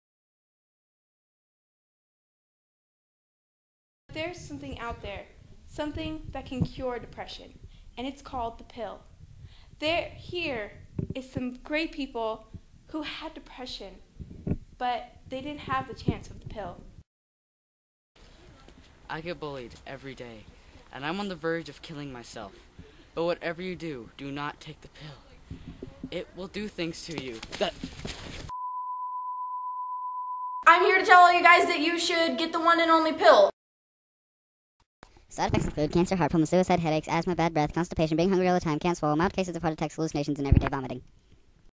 The Pill! (Official Commercail)
Script: Darby Bernoudy
Actors: Darby Bernoudy, Annika Novak, Vincent Williams, and Kortney Ricketts
Location: Golden Sierra High School
Narrator: Vincent Williams
Camera: Vincent Williams, Annika Novak, and Darby Bernoudy
Enjoy! And DO NOT TAKE THE PILL!